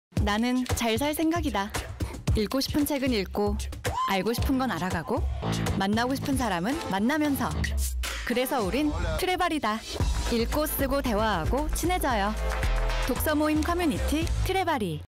[포커스미디어] 트레바리_브랜딩_여자편
입주민 여러분,
혼자가 아니라 함께
읽고 쓰고 대화하고 친해질 수 있는 곳을 찾으시나요?

그럼 트레바리 하세요!

독서모임 커뮤니티 트레바리에서
더 많은 사람들과 이야기하세요!